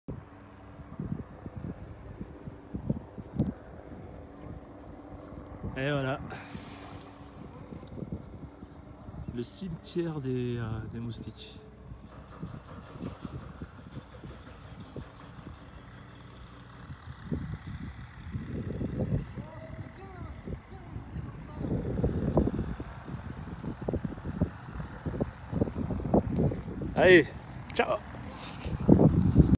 Colonel Z - Fly Synthesis Texan - La saison des moustiques suicidaires est ouverte
La saison des moustiques suicidaires est ouverte !

Season of suicidal mosquitoes is open !
Fly Synthesis Texan
Pistes où j'ai décollé et atterri en aviation privé (Avion & ULM)
Vidéo réalisée avec des caméras sportives (Go Pro HD & Garmin VIRB).
Video made with sports cameras (Go Pro HD & Garmin VIRB).